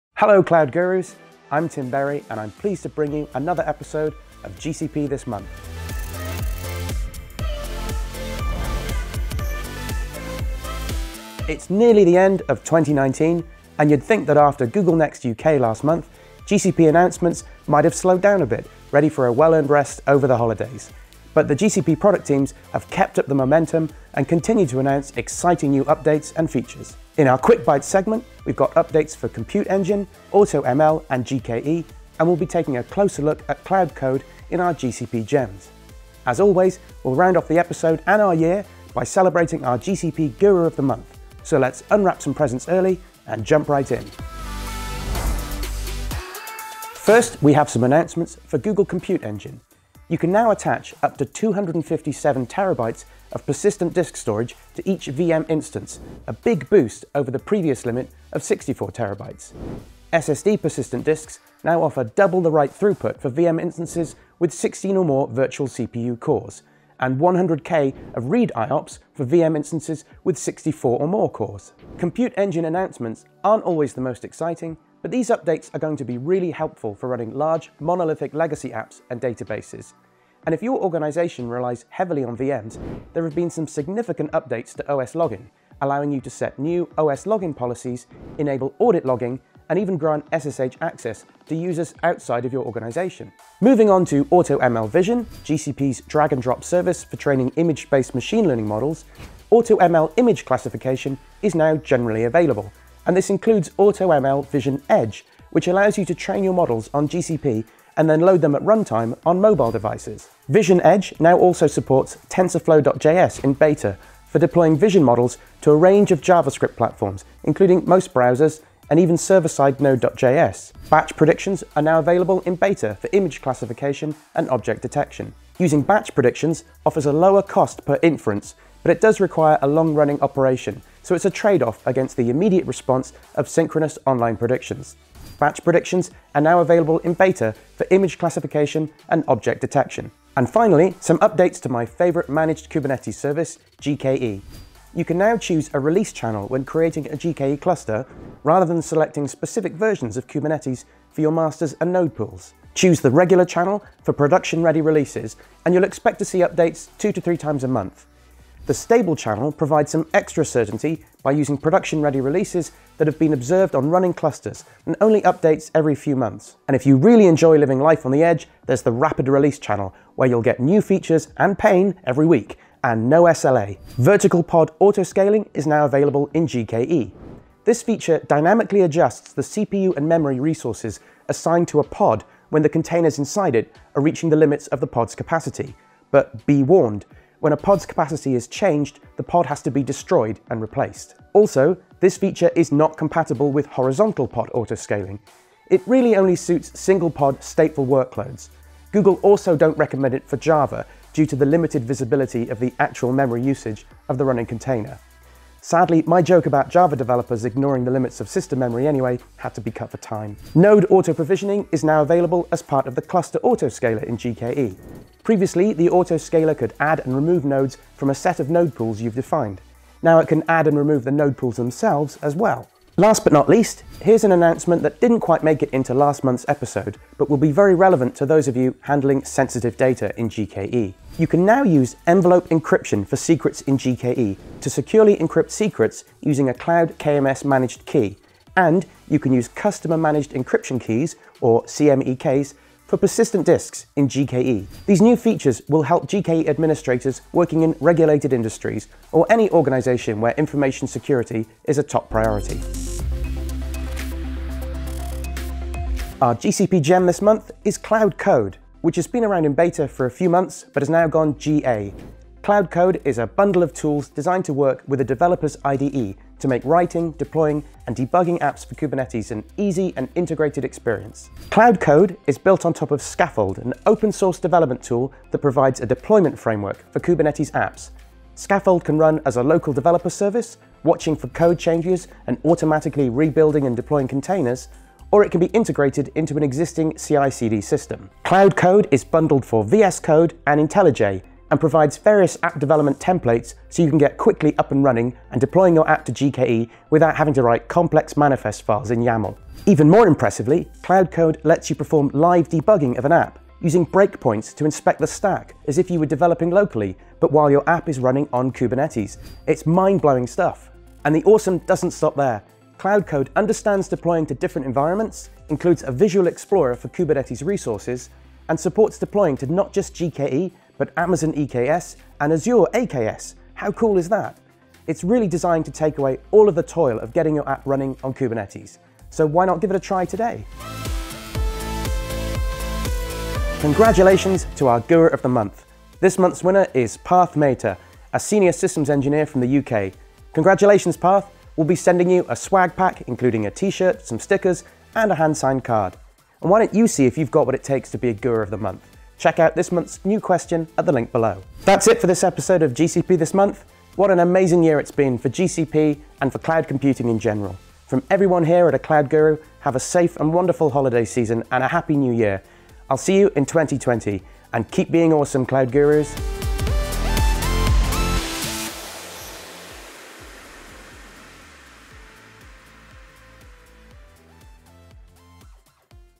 GCP gifts for Compute Engine, AutoML and GKE
We've reached the end of the year, but GCP still have some more gifts for us in the form of updates for Compute Engine, AutoML and GKE. In this episode, our host Tim Berry also takes a closer look at Cloud Code entering GA and explains exactly how this bundle of tools can make writing, deploying and debugging apps for Kubernetes an easy and integrated experience. And finally, we announce our last Guru of the Month winner for 2019!